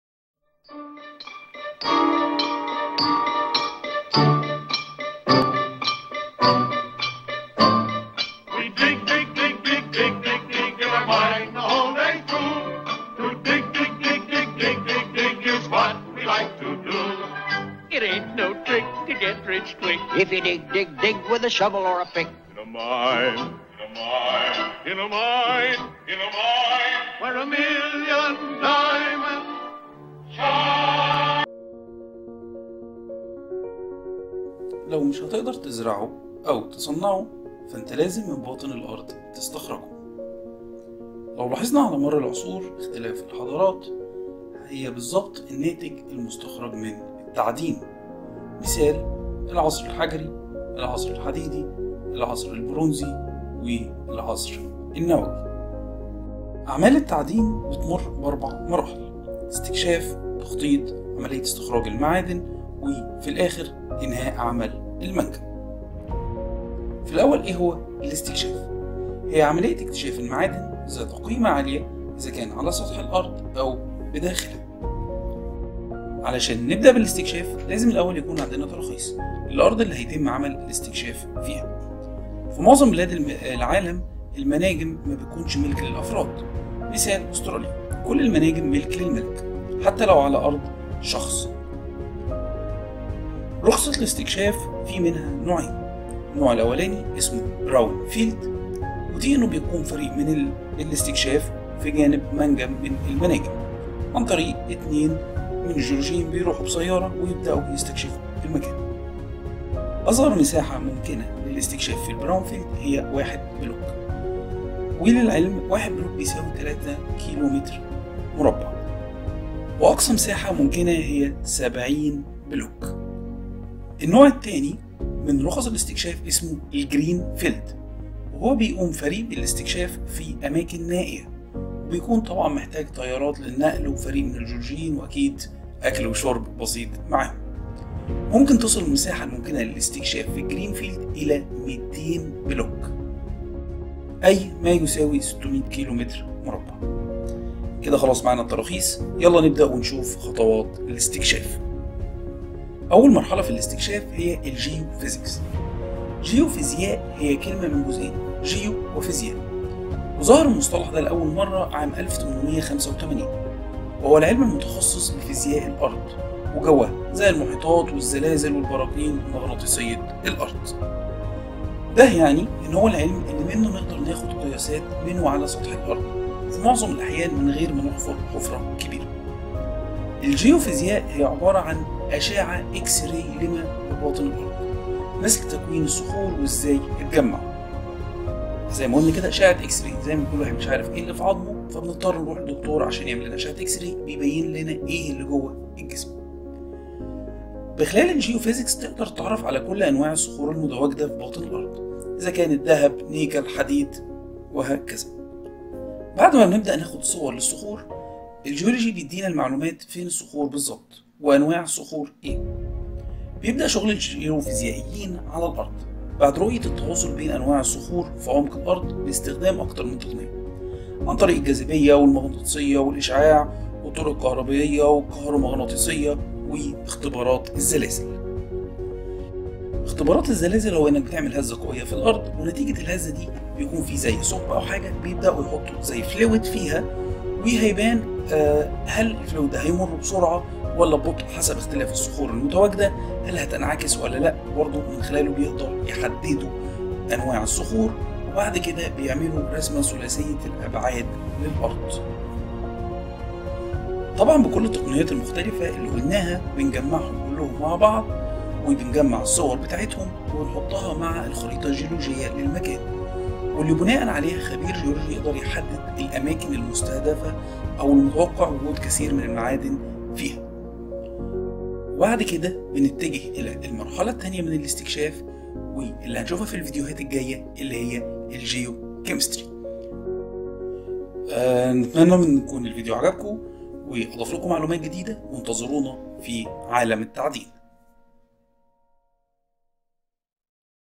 Geophysics of mining
in this Videos we will explain the Exploration process - Geophysics process